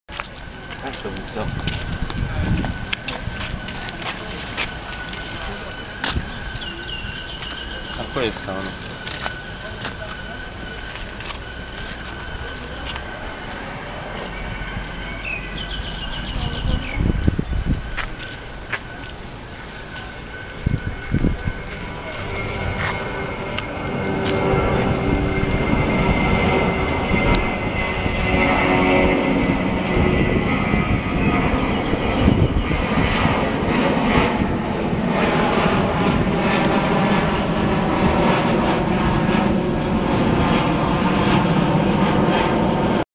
Thailandia, panoramica del buddha temple a Koh Samui.
Siamo a Koh Samui un'isola a sud della Thailandia, andiamo a visitare il famoso tempio del Big Buddha.